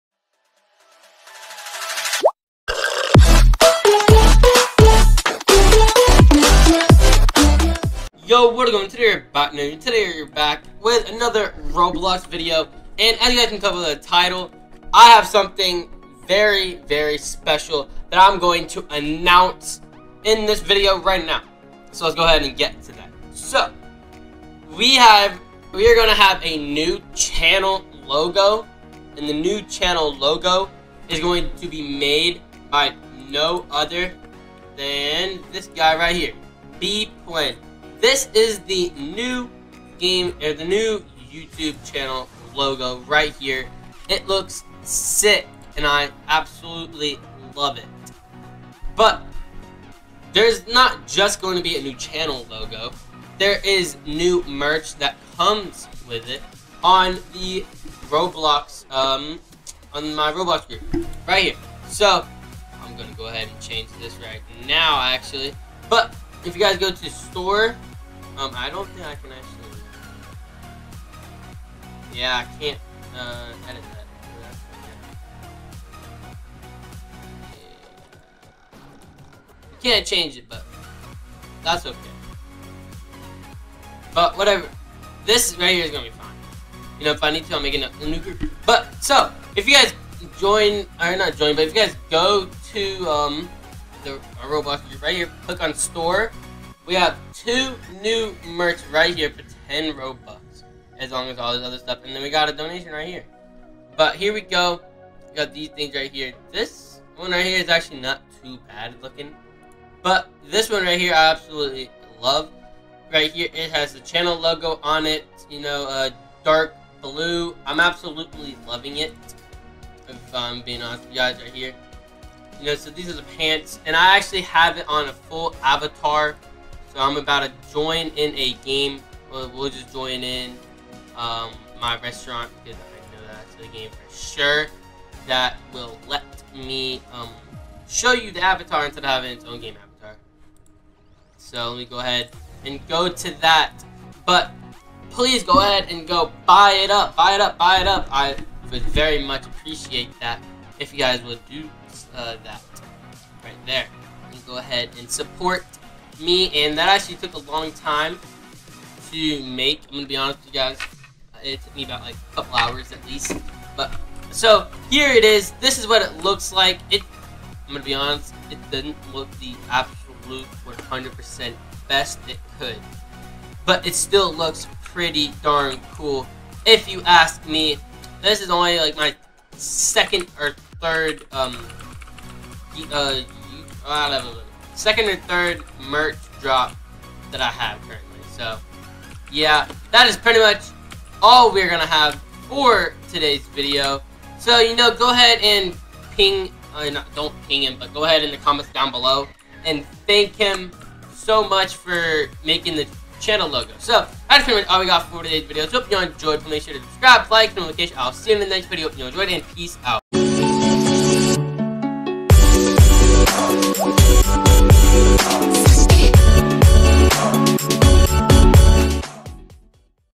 *NEW* Channel Logo + MERCH! (Roblox)
In this video I showed you all the new Roblox Merch in the Roblox Group & the new channel logo! Massive thanks for Bpwen for the logo!